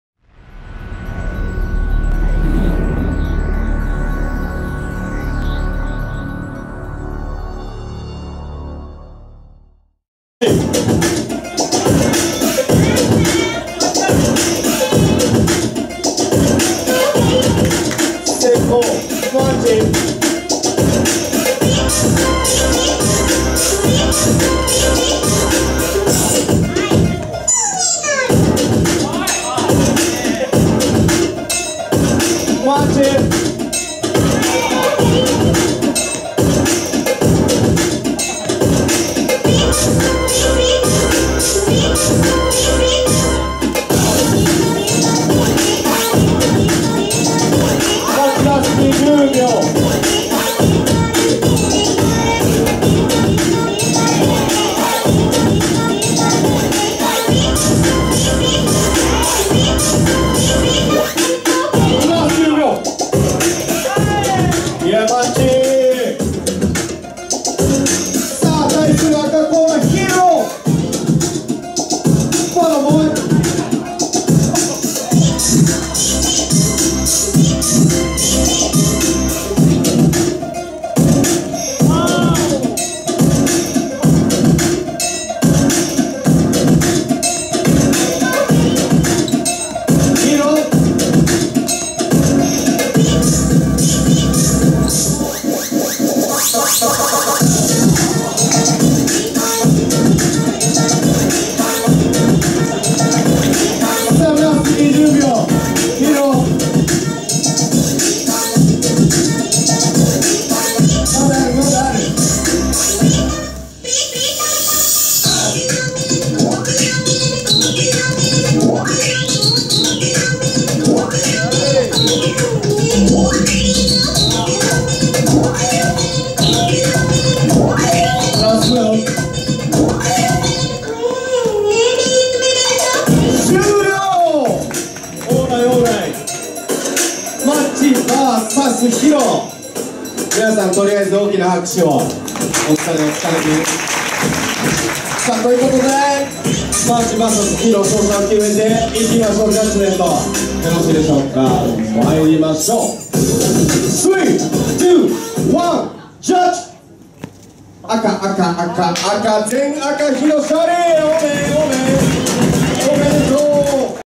DOUBLE DUTCH ONE'S 2017 SCENE 2 EAST 1st BATTLE【 Macchi vs HIRO 】
[WINNER]
HIRO

[ DJ ]
DJ Da-j / DJ TOSHI

[ TURNER ]
SHOTA
REINA

[ JUDGE ]
JUNKNESS [YUTTY KINGDOM]
TATSUYA [Waffle]
DAICHI [YUTTY KINGDOM]
SHIGE [Who is Respected]